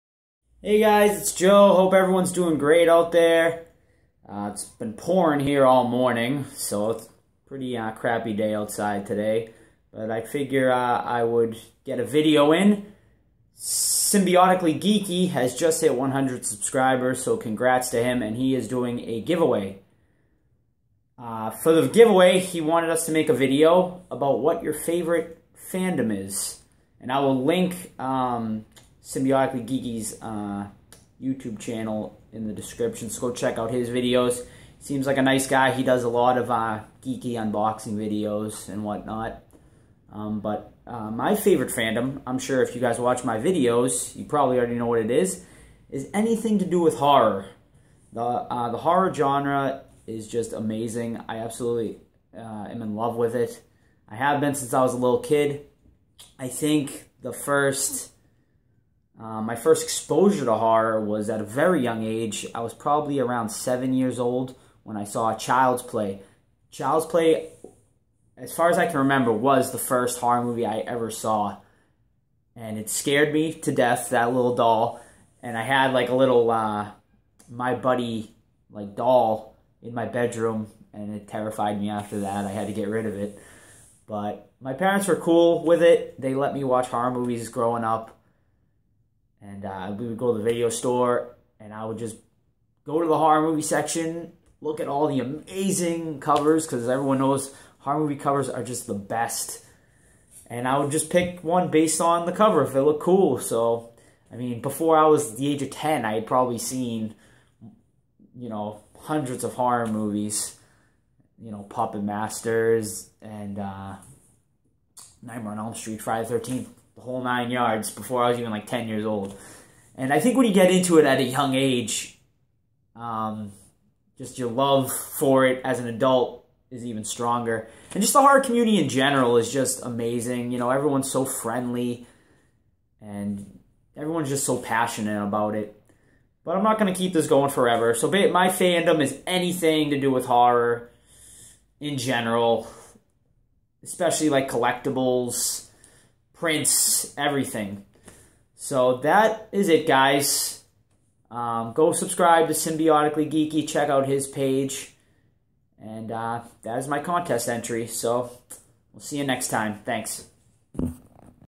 Symbiotically geeky 100 subscriber giveaway entry
Hey guys symbiotically geeky is doing a giveaway because he hit 100 subscribers go check out his channel and enter too